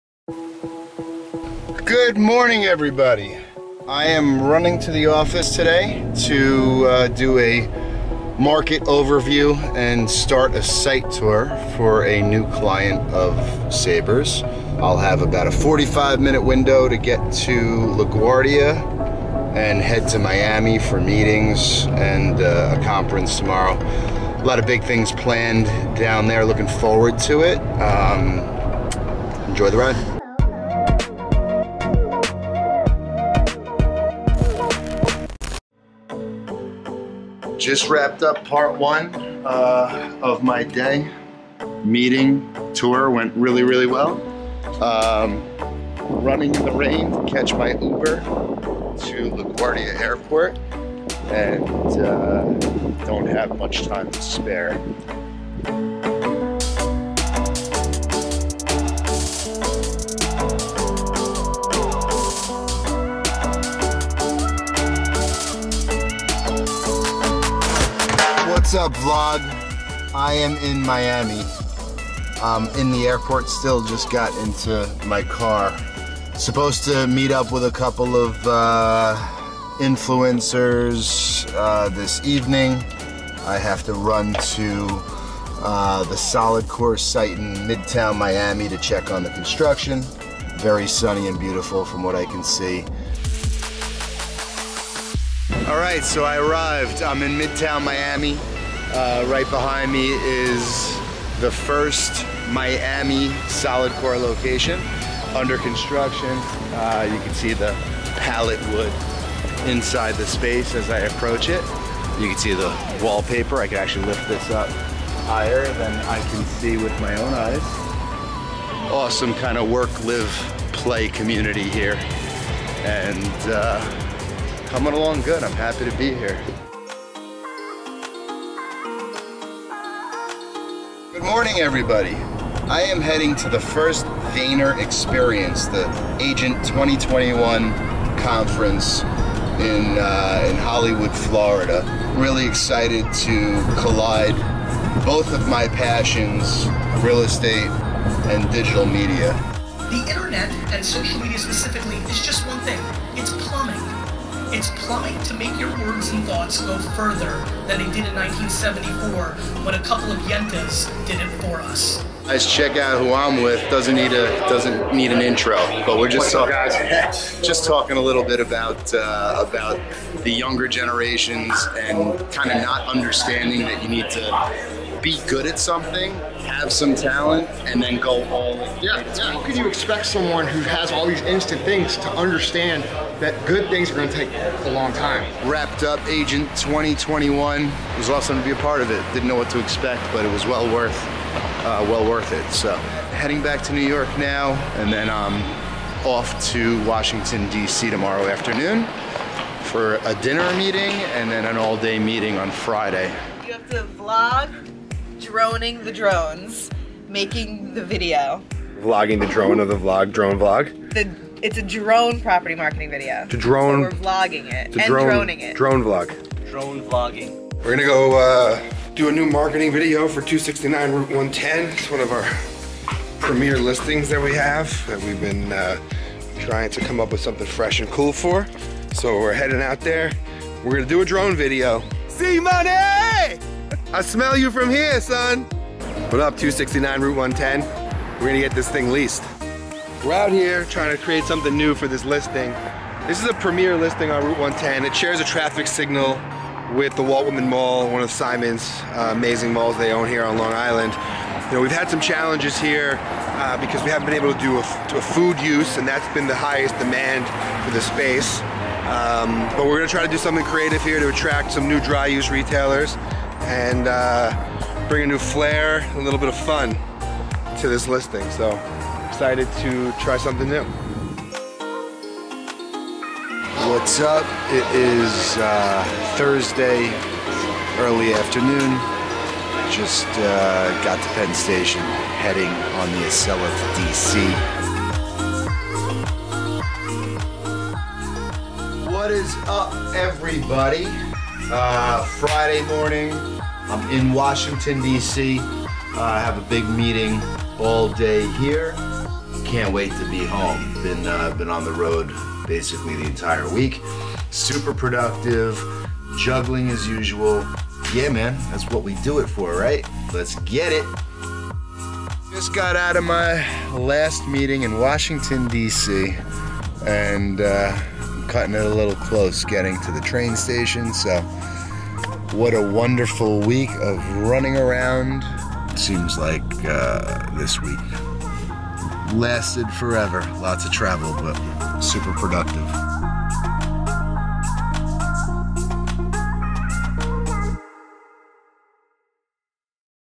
Miami, New York, and D.C. - Real | Sabre #54
▶ Jay Siano goes to Miami for meetings.  He bumps into Andy Frisella, and hears Gary Vaynerchuk deliver a keynote speech.
▶ Russel Helbling, Chris Petrone, and Sean Montazami film a property in Melville, NY.
▶ Jay wraps up the episode in D.C. for more meetings.

Find film effects we use at Videezy.com